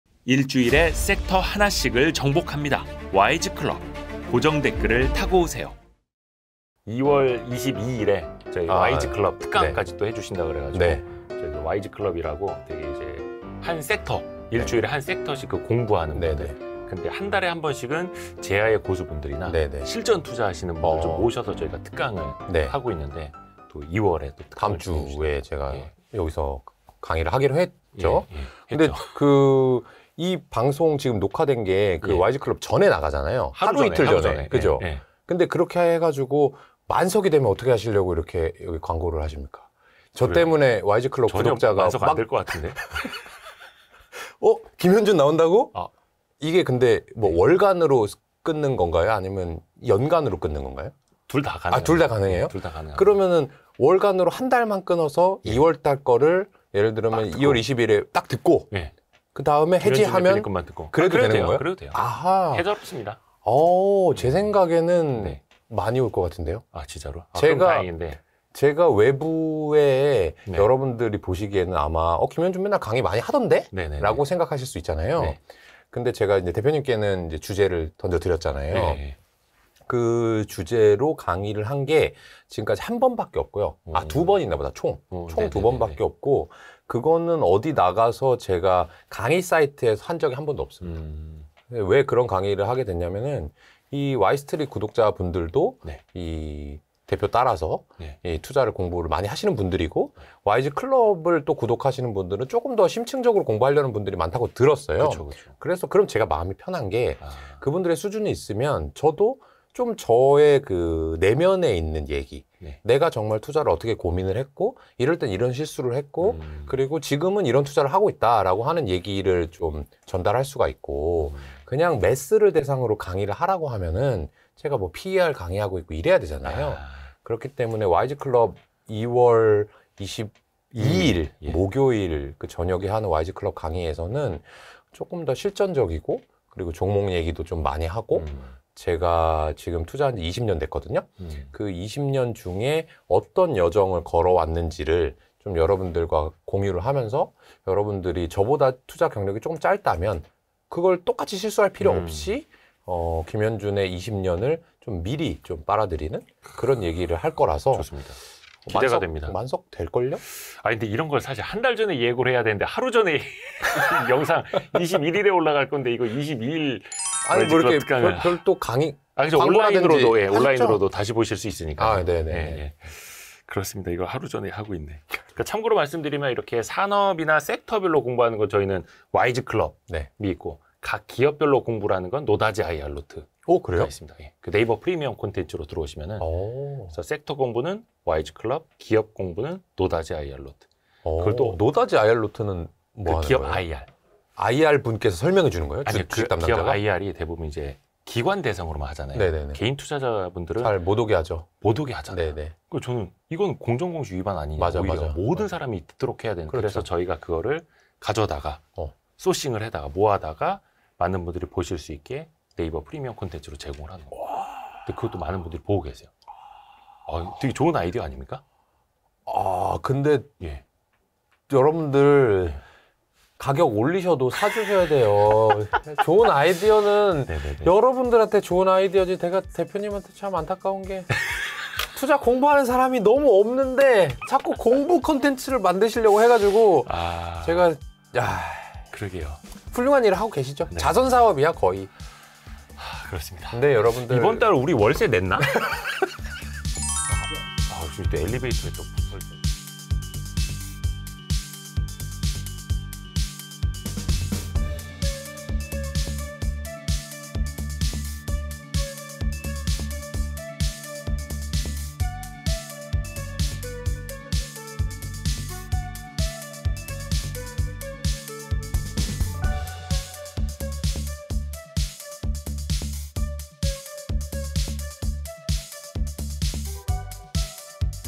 20년 투자하며 깨달은 것들, 다 알려드릴게요!...와이즈클럽에서만 가능한 투자 강의 [김현준 대표 특강 예고편]
*오프라인 30명 (2/16(금)저녁8시부터 선착순 및 추첨) / 온라인 무제한

※이 영상은 2/14에 진행된 김현준 더퍼블릭자산운용 대표와의 대담 중 일부입니다.※
"와이즈클럽 구독자들이라서 가능한 강의죠."
김현준 대표 20년 주식투자 경력의 정수를 담은 특강!
실전투자에 필요한 지혜와 종목, 섹터 이야기까지...
조기 마감이 예상되니 아래 링크로 빠르게 신청해 주세요!

"1주일에 1섹터씩 정복하기!! 와이즈클럽"

"개인에게도 IR 정보를 공평하게!! 노다지IR노트"